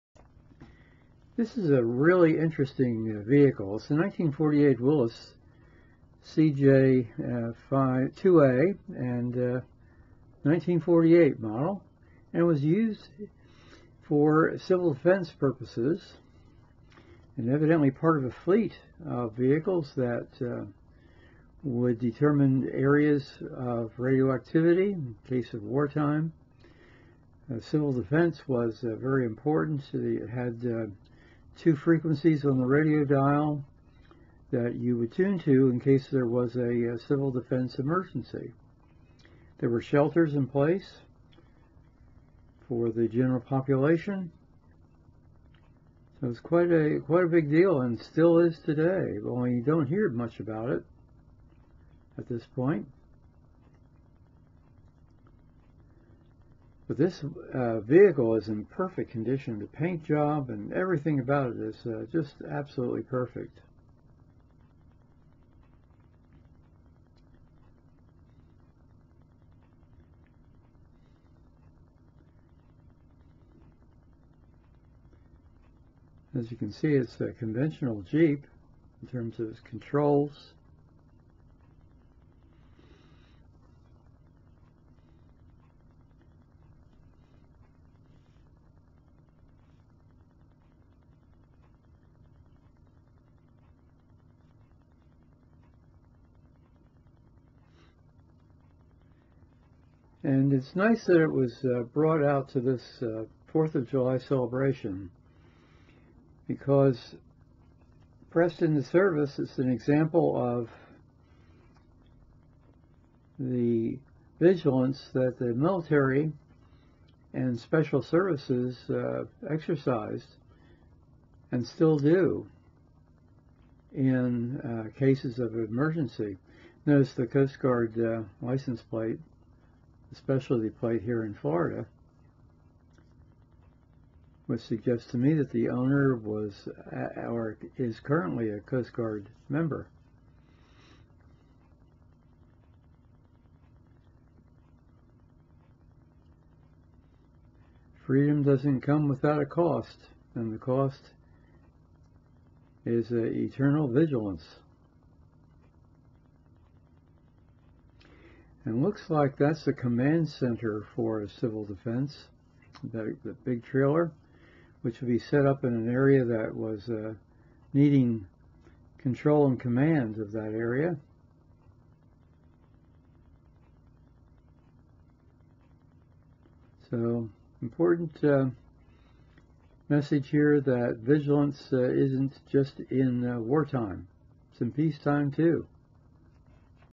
1948 Willys Jeep CJ2A Civil Defense Communications BluWht Ocoee 0704246914
HAPPY FOURTH OF JULY, EVERYONE!  This Jeep is a reminder of the important function of Civil Defense in safeguarding the general population.  It managed the shelters during the cold war and continues to provide assistance in times of natural disaster and hostilities.

Show less